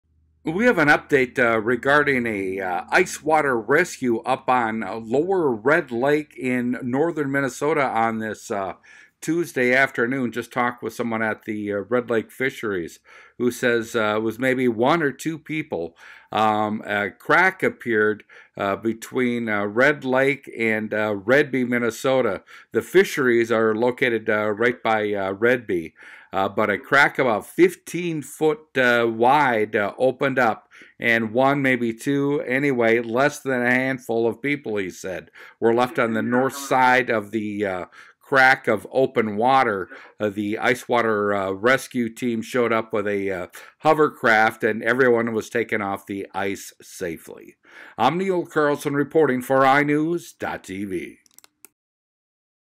UPDATE: Hovercraft Used For Ice Water Rescue On Lower Red Lake, MN
RED LAKE, MN  (iNewZ.TV) Everyone is safe, after a hovercraft was used Tuesday on Lower Red Lake to rescue fishermen, after a crack opened up.